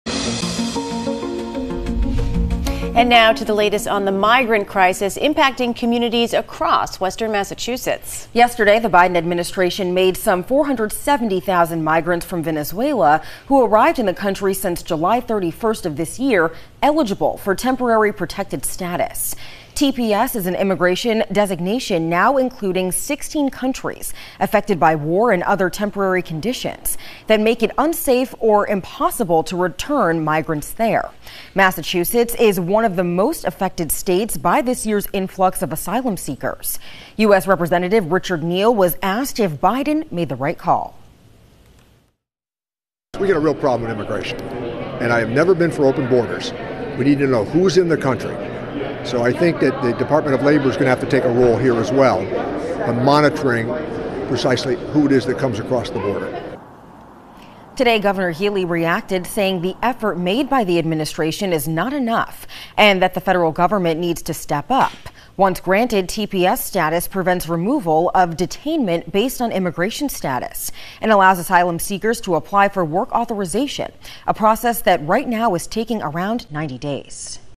Biden administration grants TPS to Venezuelan migrants
Thursday the Biden Administration made some 470,000 migrants from Venezuela who arrived in the country since July 31st of this year, eligible for Temporary Protected Status (TPS).